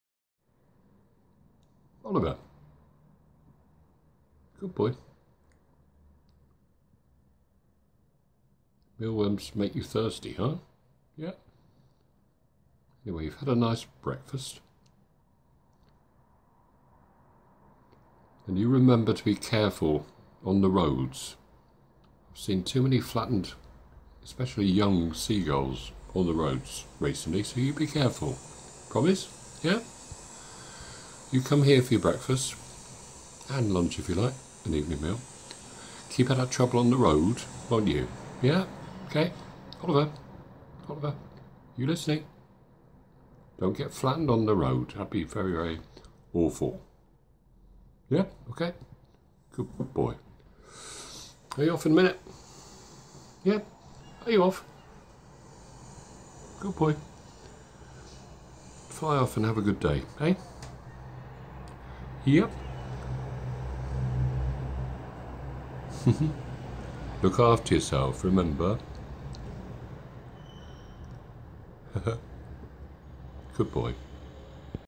Giving 'Oliver 2', 'My' Herring Gull Fledgling, Road Safety Advice - I hope he listened carefully!
'Oliver 2' is this year's fledgling of the pair of herring gulls, Sassy and Sybil, that I feed daily at my kitchen window. Here, I am giving him some road safety advice as, sadly, I've seen too many flattened gulls, especially fledglings, on the local roads recently. I hope he listened carefully 😊